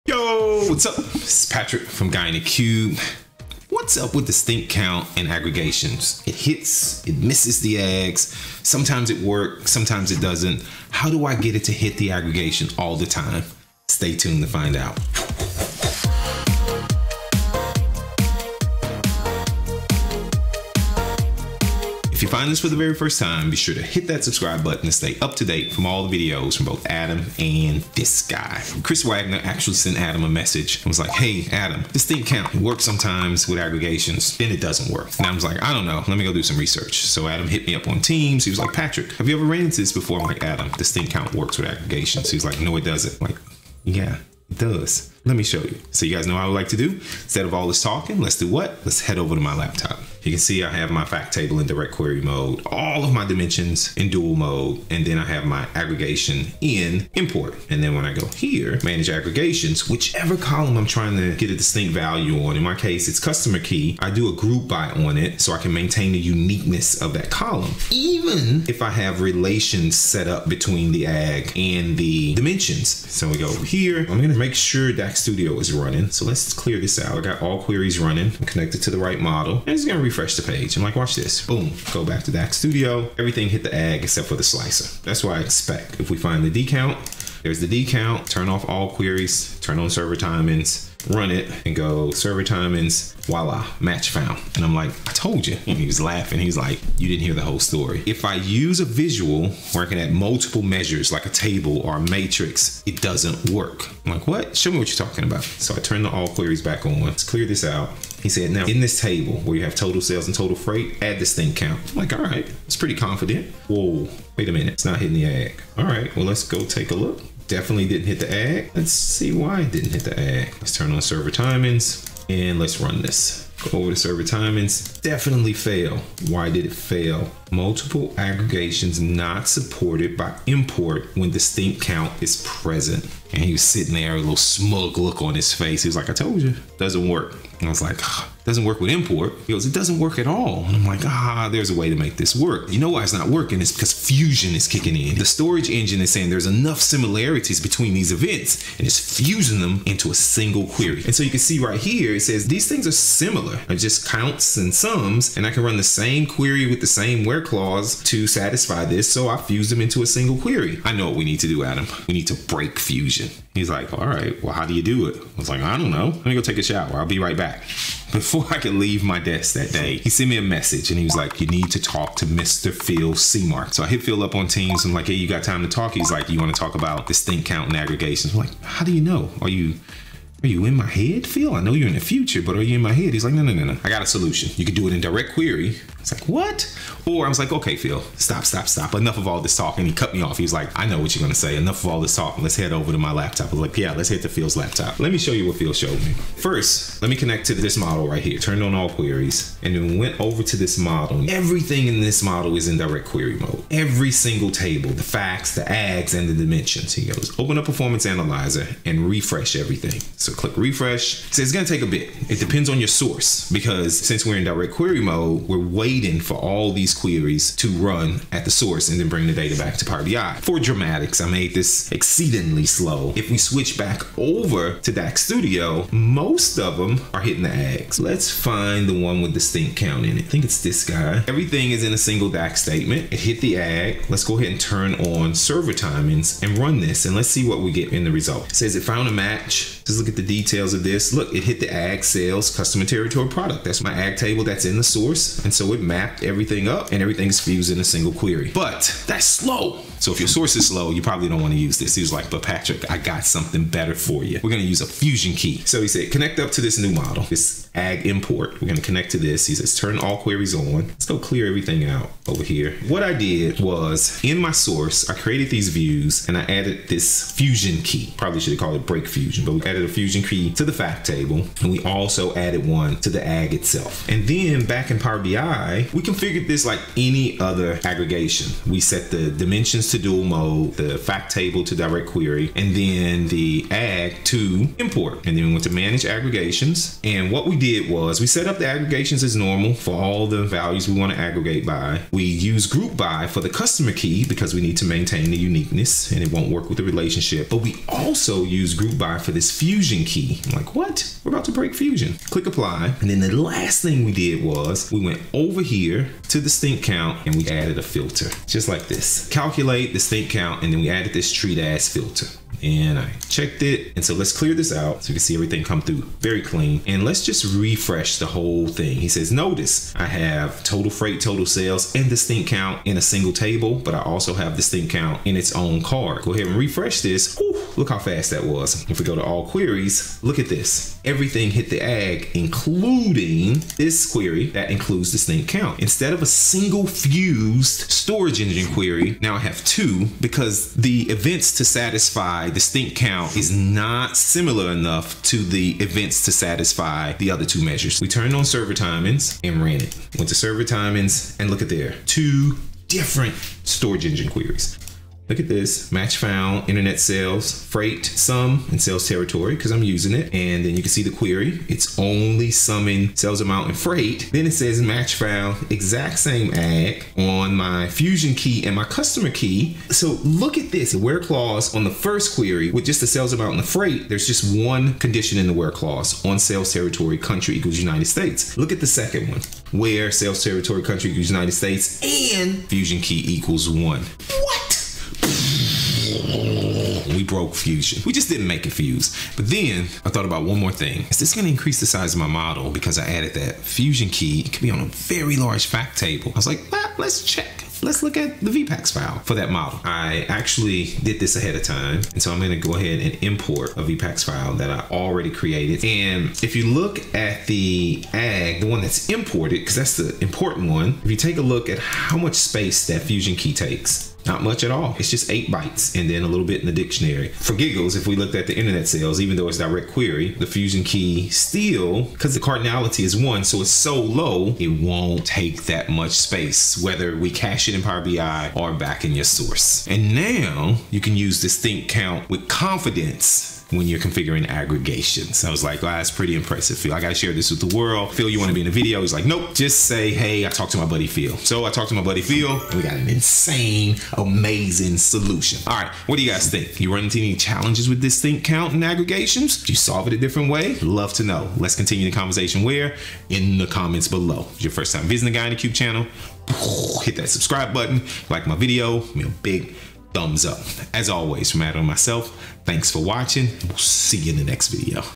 Why isn't my Agg hitting when using DISTINCTCOUNT in Power BI???
Using Aggregations in Power BI? Have some DISTINCTCOUNT in your measures? Wondering why the queries are slow and not hitting the aggs? Patrick shows you what is going on! Get ready for some DAX fusion!